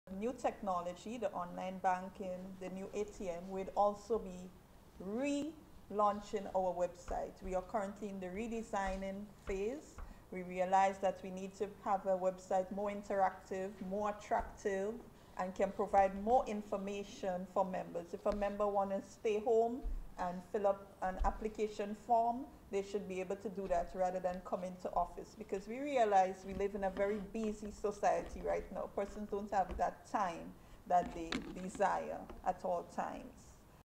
A New Look
Some good news for customers of the Communal Cooperative Credit Union Limited, as the company announced the re-designing of its online website.
Marketing Officer at the Communal Cooperative Credit Union Chinnel Andrews says this new website will be delivering better experience to their customers.